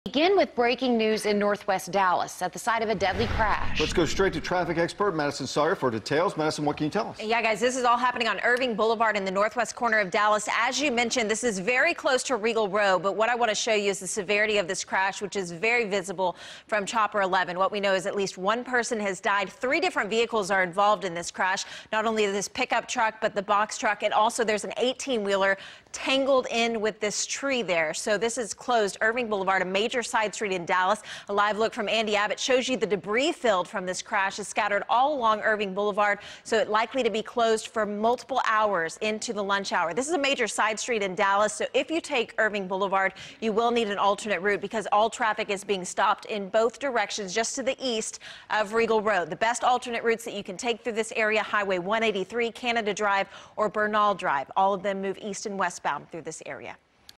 Deadly Crash In Northwest Dallas
Madison Sawyer explains what happened and how to get around the backup.